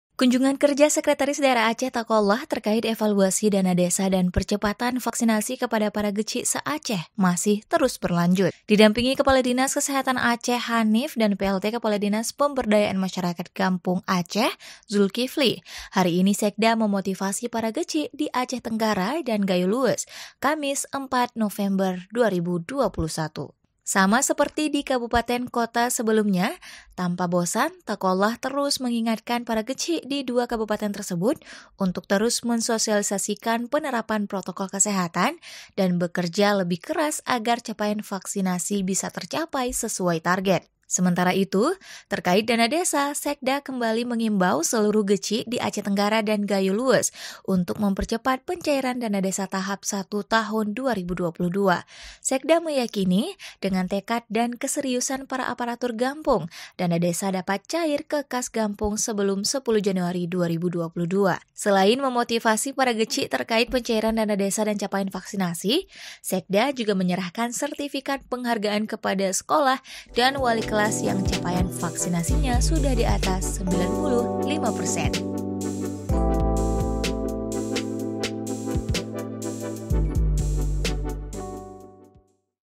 Evaluasi Dana Desa dan Percepatan Vaksinasi Berlanjut ke Aceh Tenggara dan Gayo Lues
Kunjungan kerja Sekretaris Daerah Aceh Taqwallah, terkait Evaluasi Dana Desa dan Percepatan Vaksinasi kepada para keuchiek se-Aceh masih terus berlanjut.

Didampingi Kepala Dinas Kesehatan Aceh Hanif dan Plt Kepala Dinas Pemberdayaan Masyarakat Gampong Aceh Zulkifli, hari ini Sekda memotivasi para keuchiek di Aceh Tenggara dan Gayo Lues, Kamis (4/11/2021).

Sama seperti di kabupaten/kota sebelumnya, Tanpa bosan Taqwallah terus mengingatkan para keuchiek di dua kabupaten tersebut untuk terus mensosialisasikan penerapan protokol kesehatan dan bekerja lebih keras agar capaian vaksinasi bisa tercapai sesuai target.

Sementara itu, terkait Dana Desa, Sekda kembali mengimbau seluruh keuchiek di Aceh Tenggara dan Gayo Lues untuk mempercepat pencairan Dana Desa tahap 1 tahun 2022. Sekda meyakini, dengan tekad dan keseriusan para aparatur gampong, Dana Desa dapat cair ke kas gampong sebelum 10 Januari 2022.

Selain memotivasi para keuchiek terkait pencairan Dana Desa dan capaian vaksinasi, Sekda juga menyerahkan sertifikat penghargaan kepada sekolah dan wali kelas yang capaian vaksinasinya sudah di atas 95 persen.